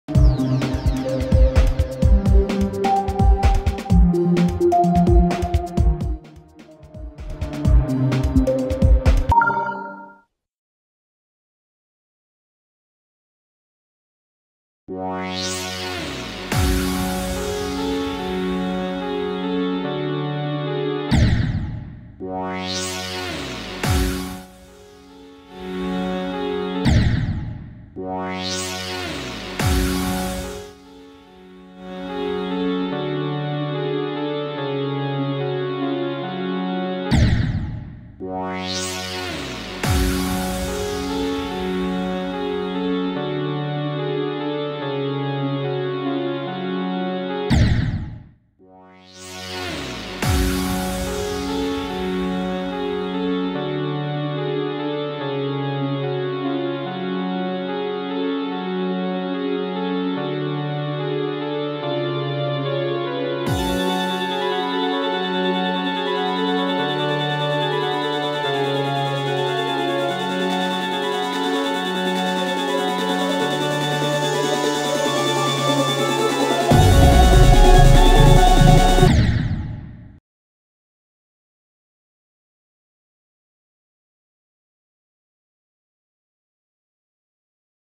This is why I’ve haven’t been uploading(read description)
I’ve been working on these levels for a few months and I’ve been working gacha life it’s taking for ever pls forgive me for my disappearance I will try to upload more 😔